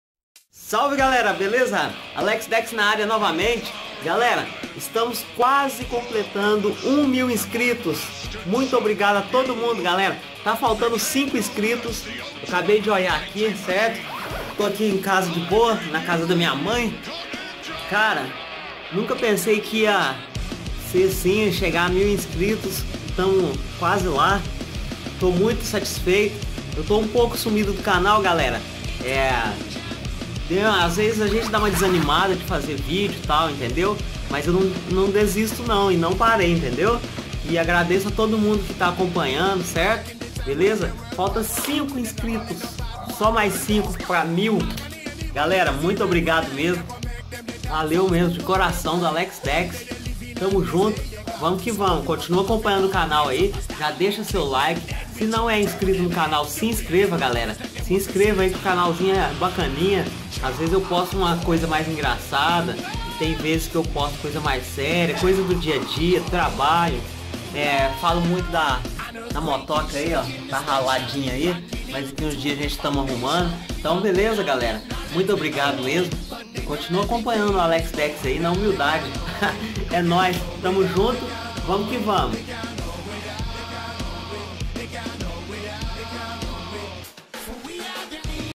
Agradecimento,obrigado a todos mesmo!!!deixem seu like e se inscrevam.
Salve galera,vídeo breve,agradecendo a todos os inscritos.
Estamos quase lá,quase mesmo chegando a 1 K.
Vlw e vamos que vamos sempre!!!